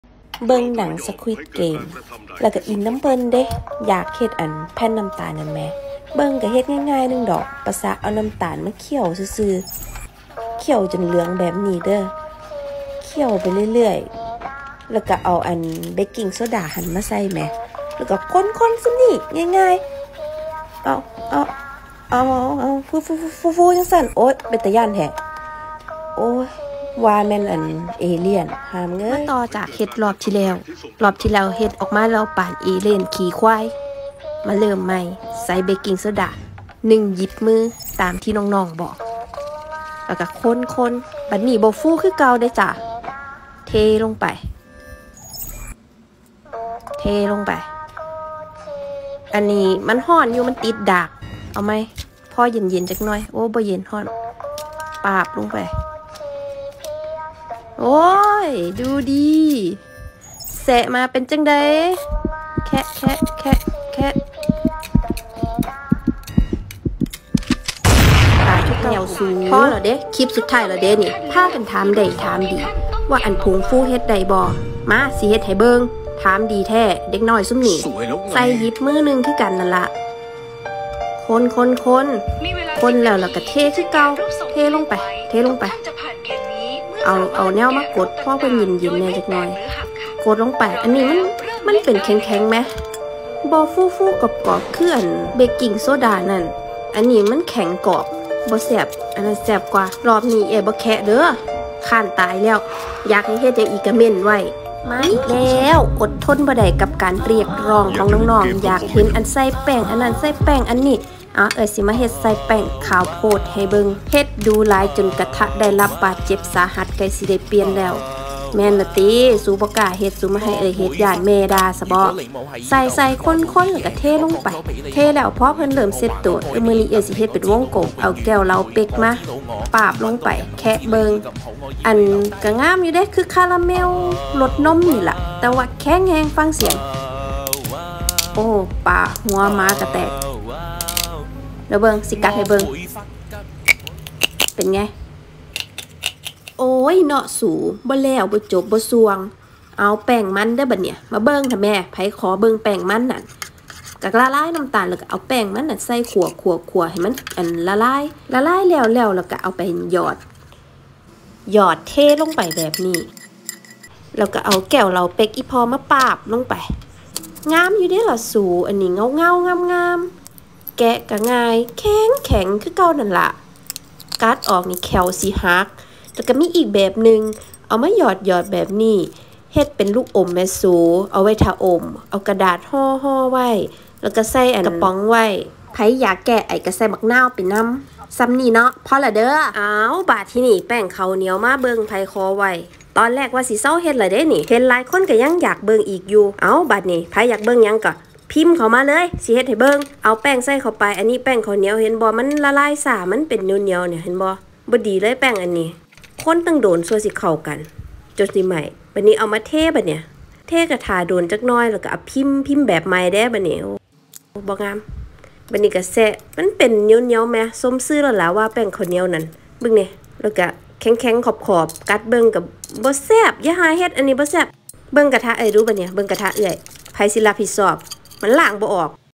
ทดสอบทำDalgona candy ทำจากแป้งชนิดต่างๆ ผลออกมาต่างกันยังไงบ้าง
คลิปนี้รวมคลิปจากTiktok เป็นการทดลองทำแผ่นน้ำน้ำจากซีรี่ย์เรื่อง Squid game โดยการทำจากวัตถุดิบต่างกันผลออกมาก็ต่างกันไป